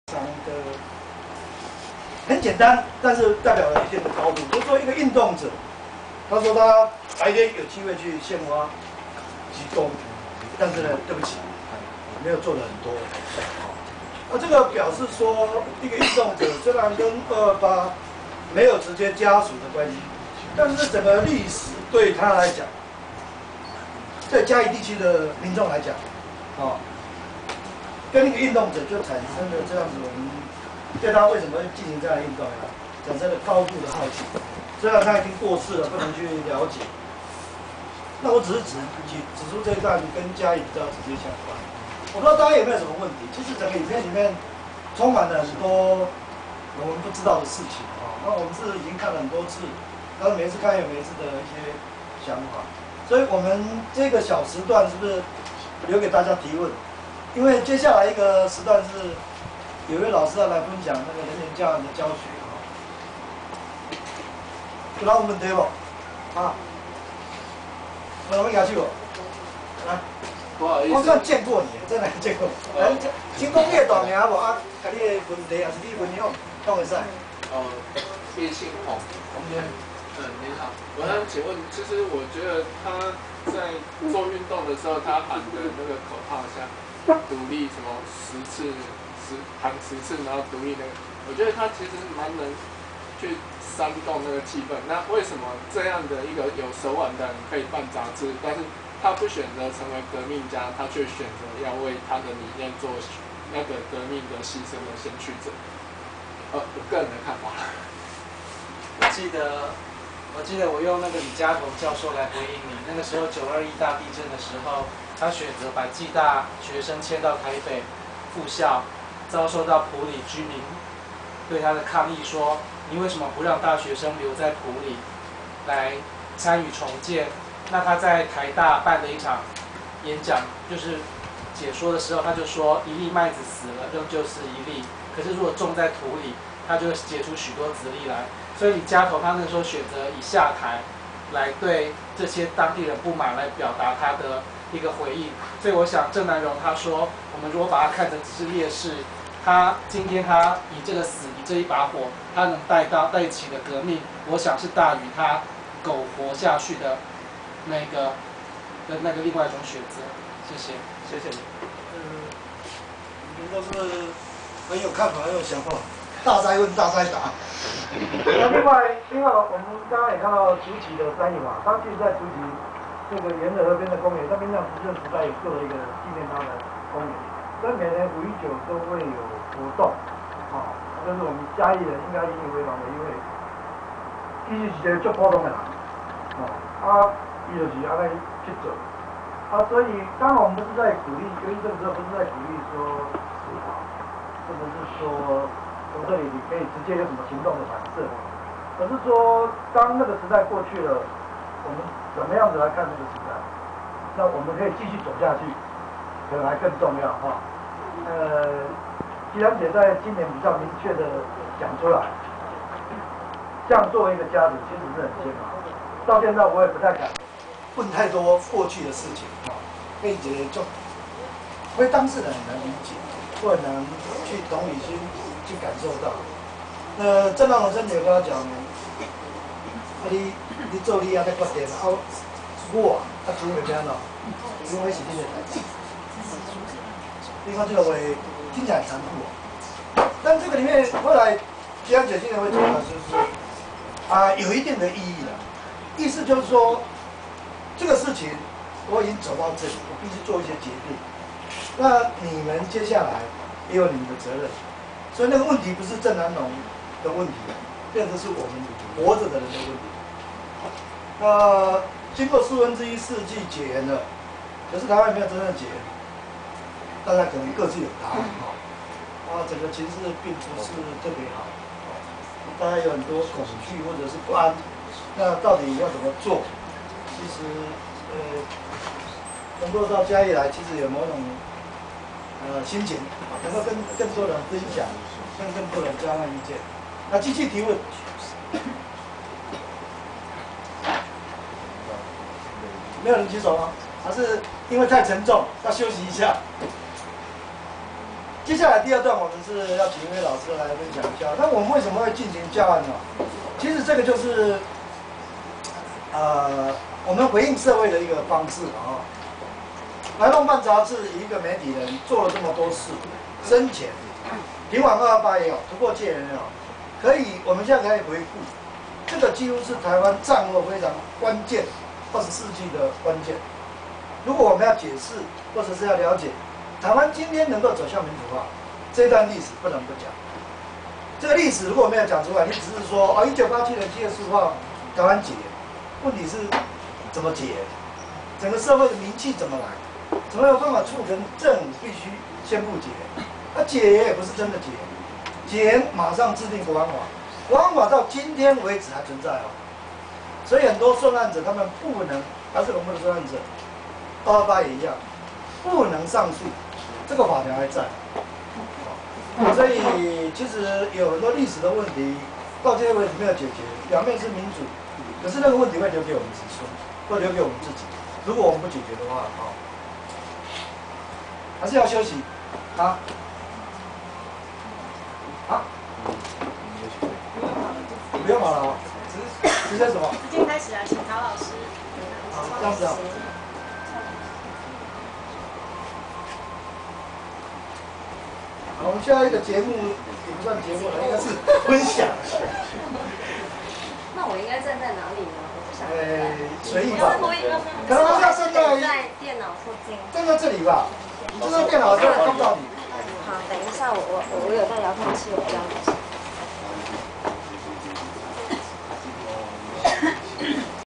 曹欽榮：鄭南榕紀錄片映後分享
「鄭南榕」不能單純以「建國烈士」來定義

他為了呼喊台灣獨立背後的【百分之百言論自由】
為了喚醒仍舊被【內心的警總】綑綁住的台灣人民
為了不讓受拘束的肉體箝制住自由的心靈

自焚，是他當下的選擇